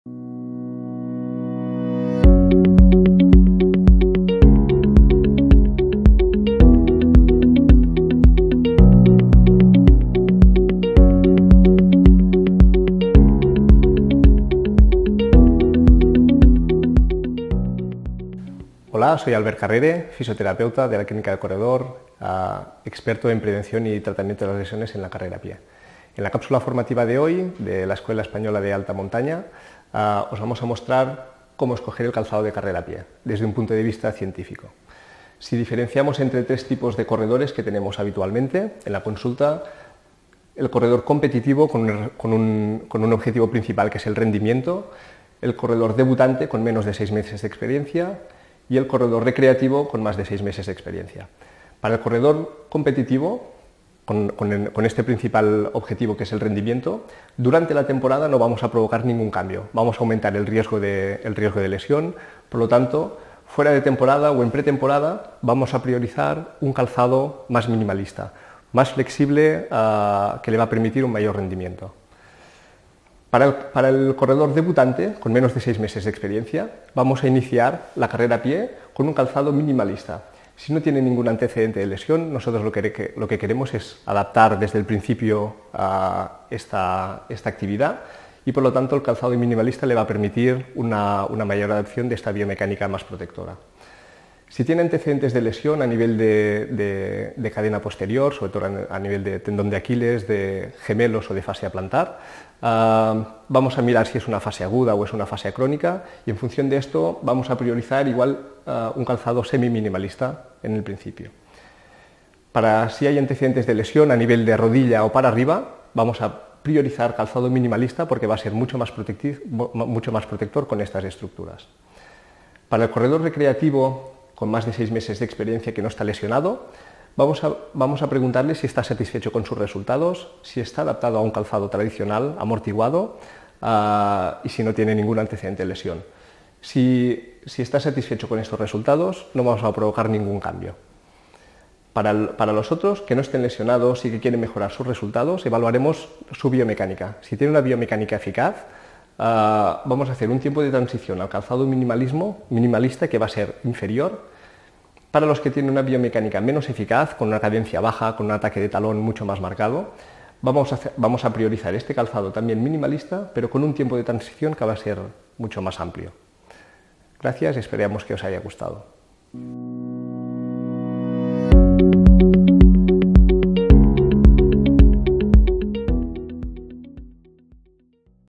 Cómo escoger el calzado de carrera por montaña (I)
En esta Cápsula Formativa, Albert Carrère, fisioterapeuta experto en prevención y tratamiento de lesiones en la carrera a pie, nos da interesantes recomendaciones para la elección del calzado en la por montaña.

Cápsula realizada en colaboración con la Clínica del Corredor.